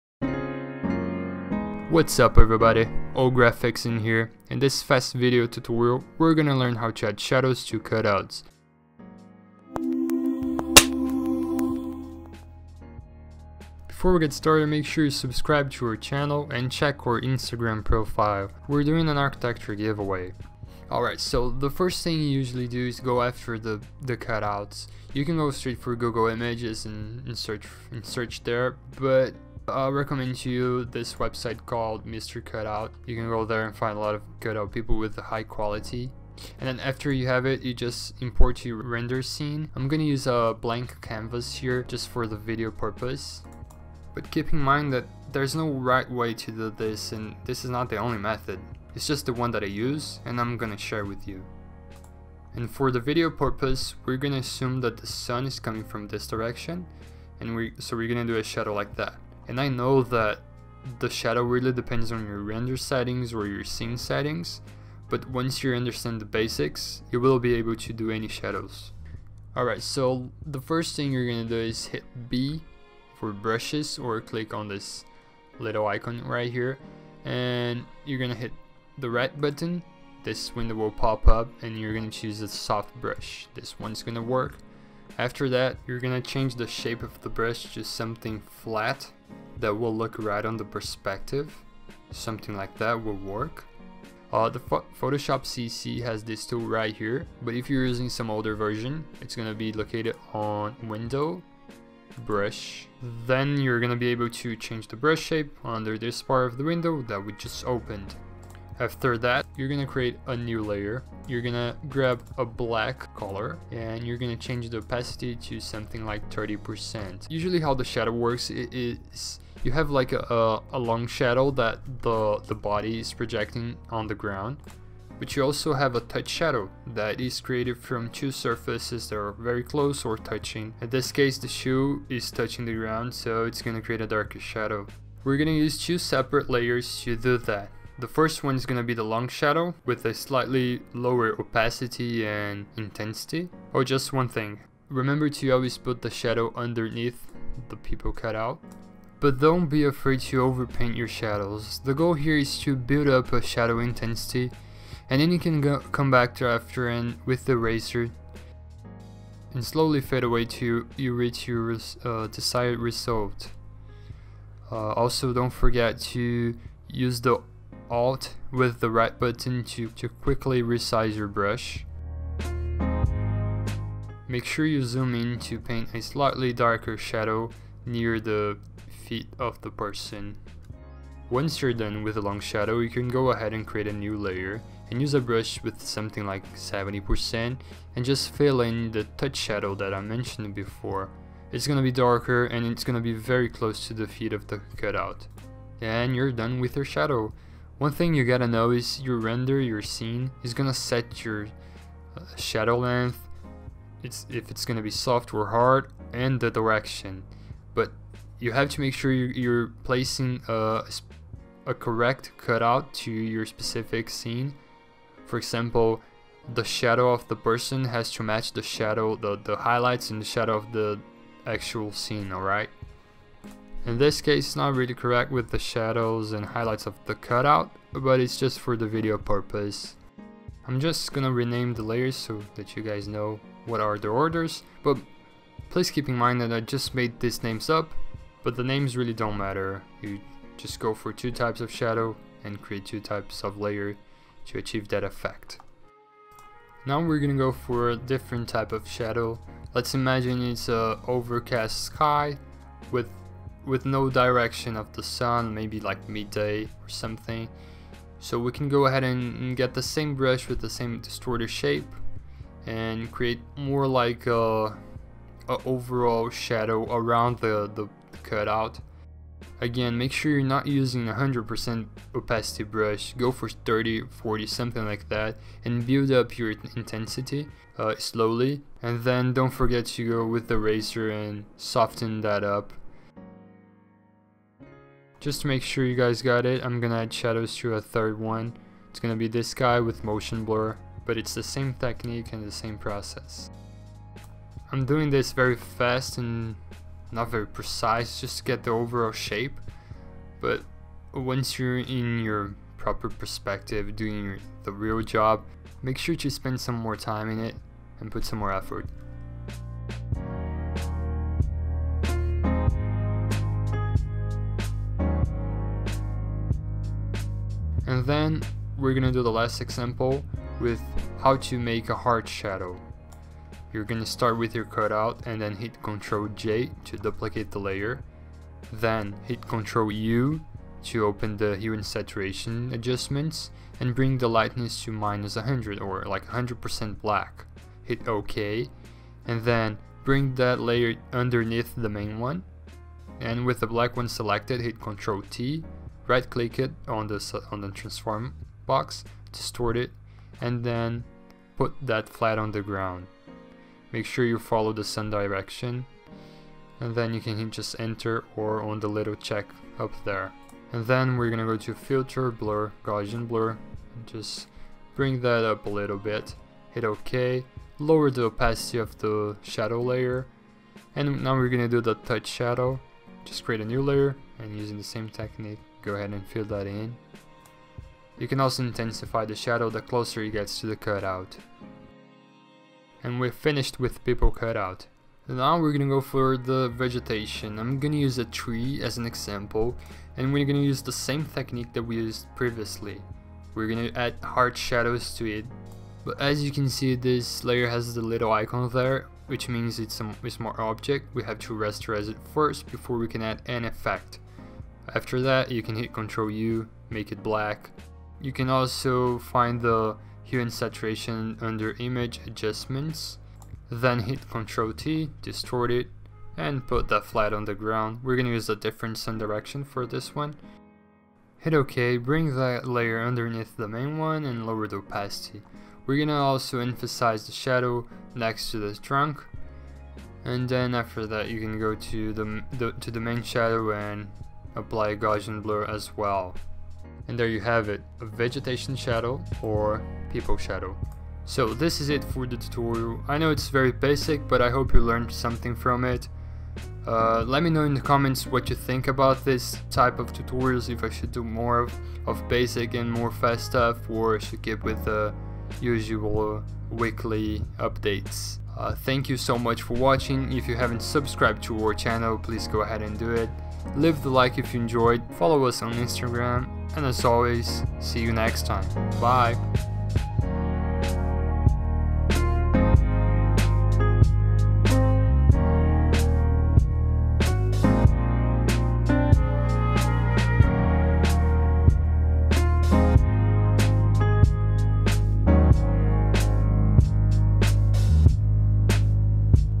How to Add Shadows to Architecture Cutout
OU Graphics has changed to Upstairs! The channel is still managed by the same people, we're just improving the identity.

Welcome to a basic tutorial! In this video, you will learn how to add shadows to Cutouts in Adobe Photoshop! This is a short video showing some very useful features that I use to fast insert people or vegetation in a render. Please keep in mind that there are several ways to do this, but I'm sharing my way of it.
This is a beginner level tutorial, if you already know how to do this, stick around and you might pick up some tips and tricks!